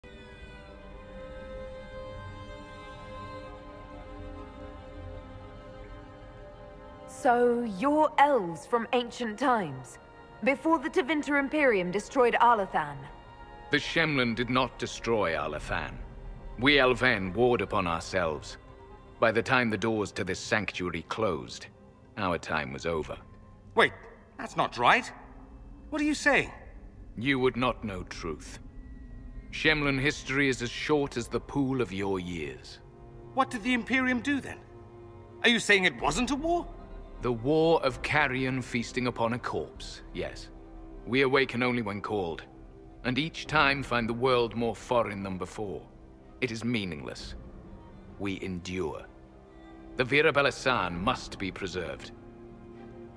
Tevinter didn't "destroy" Arlathan
Abelas contends that the elves made war upon themselves and that Tevinter was merely scavengers looting what remained.